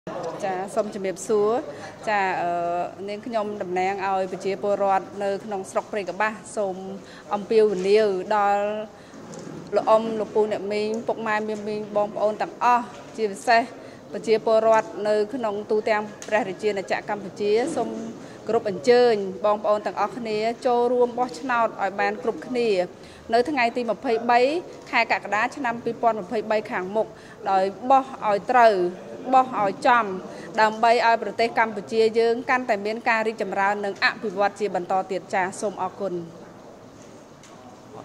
លោកស្រី បាន ឌី អភិបាលរងស្រុកព្រៃកប្បាស ខេត្តតាកែវ អំពាវនាវដល់ប្រជាពលរដ្ឋ ដែលមានឈ្មោះបោះឆ្នោតទៅចូលរួម
[WATCH] លោកស្រី បាន ឌី អភិបាលរងស្រុកព្រៃកប្បាស ខេត្តតាកែវ អំពាវនាវដល់ប្រជាពលរដ្ឋ ដែលមានឈ្មោះបោះឆ្នោត ទៅចូលរួមបោះឆ្នោតនៅថ្ងៃទី២៣ ខែកក្កដា ឆ្នាំ២០២៣ ខាងមុខឱ្យបានគ្រប់ៗគ្នា។